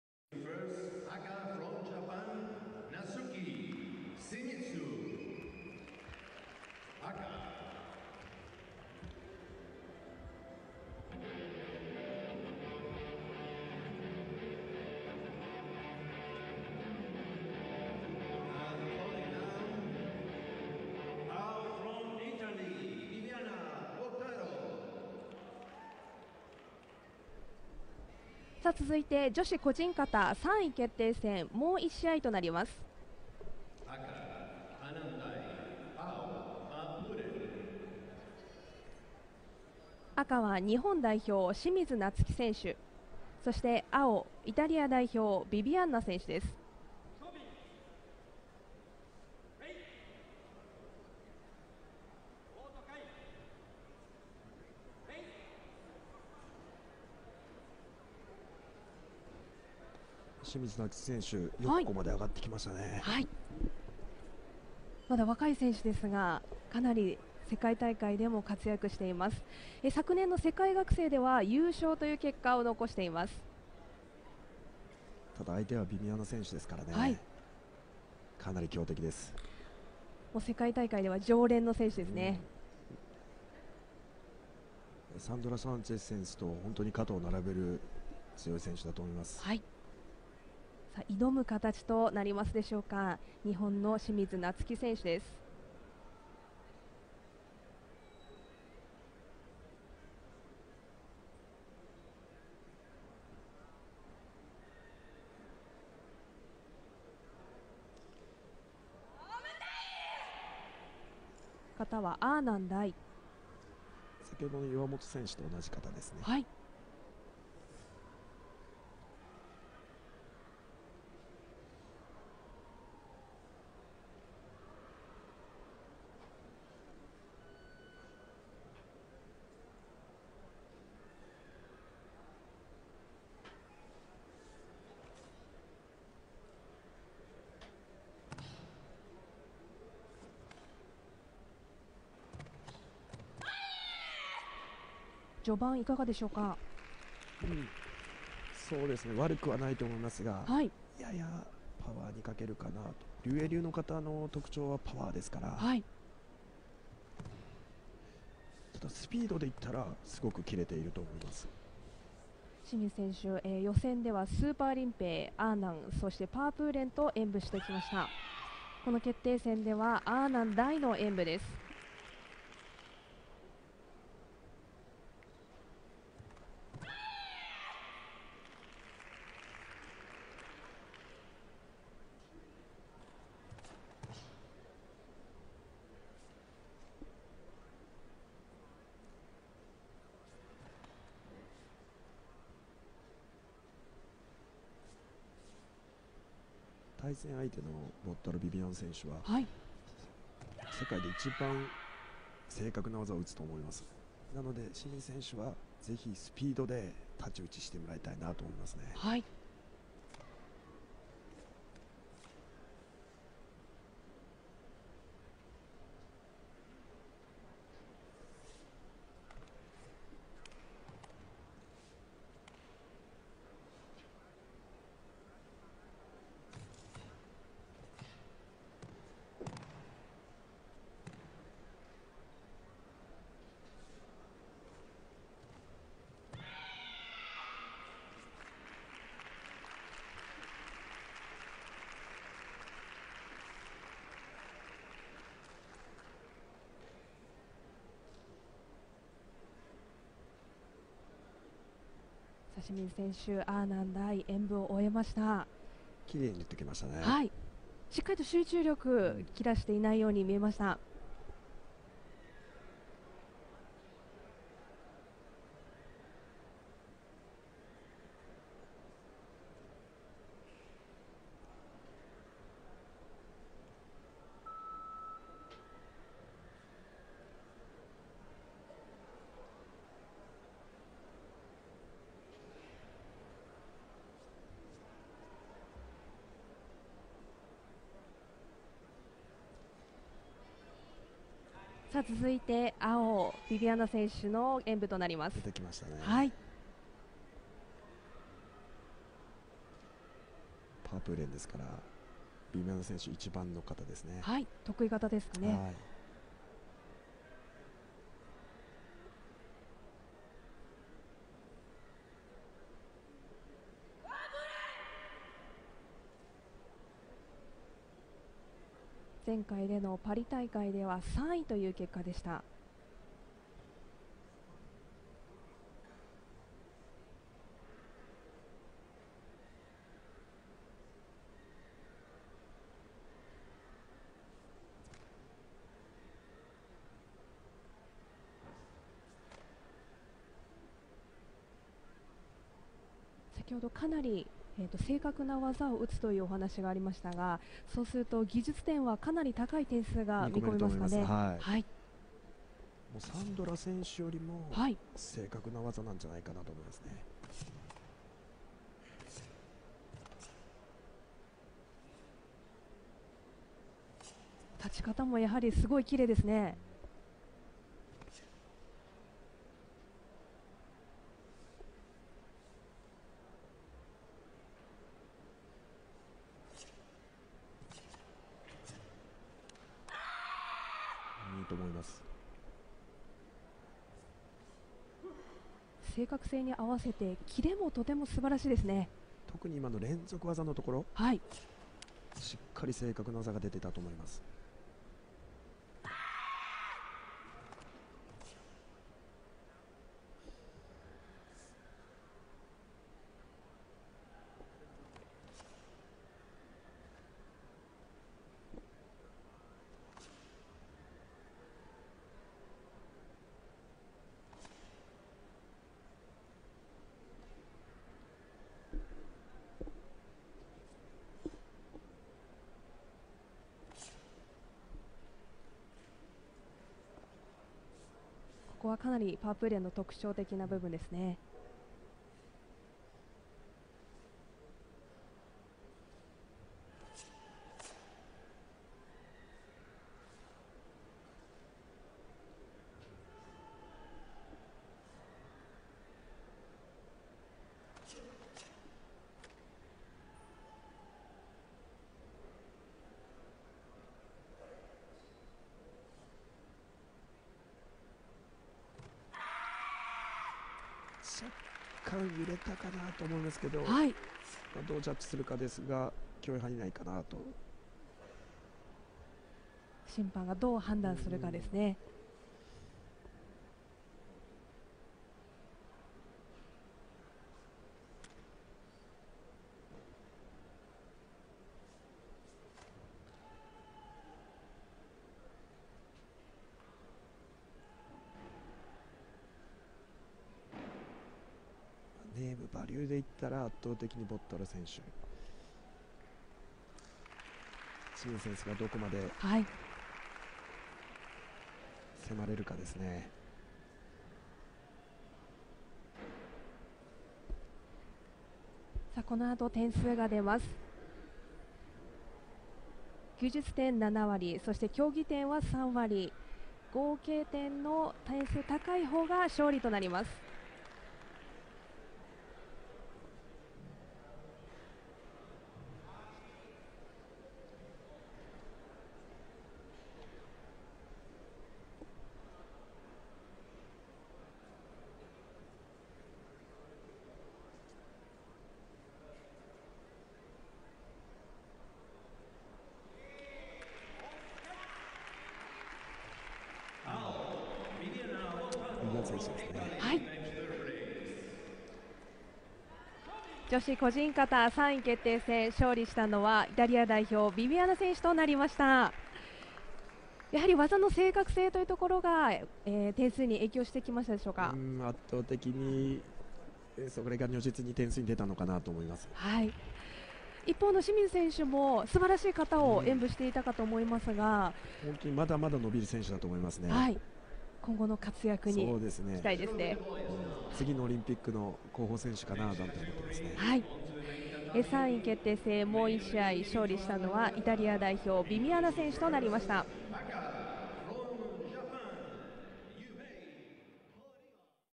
Karate Bronze -Natsuki Shimizu (JPN) vs Viviana Bottaro (ITA)
Natsuki Shimizu (JPN) vs Viviana Bottaro (ITA) - Karate 1, Dubai 2019 - Bronze, Female Kata